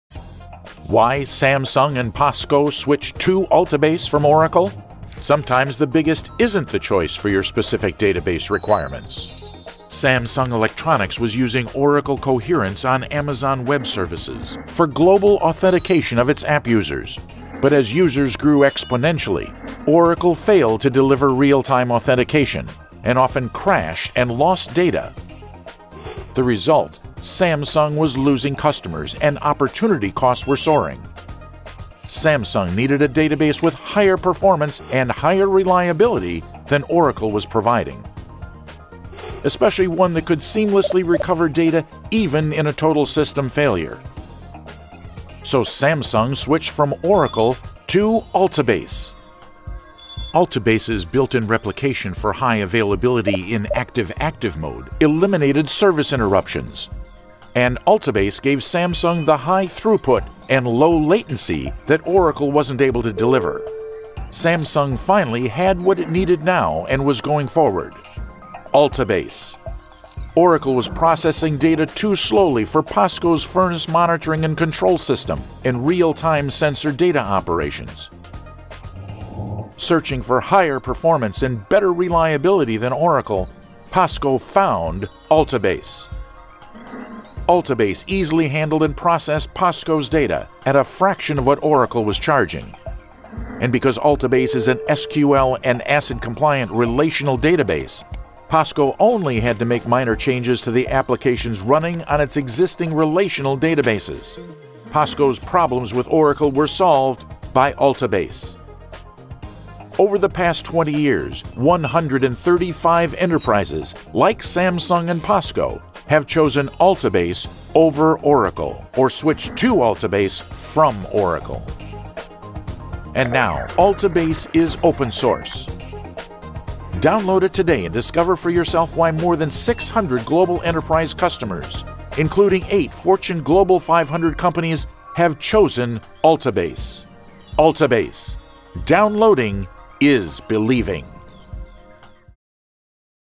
Why enterprises switch to Altibase from Oracle?
Altibase matches Oracle function for function except for one: Altibase is open source.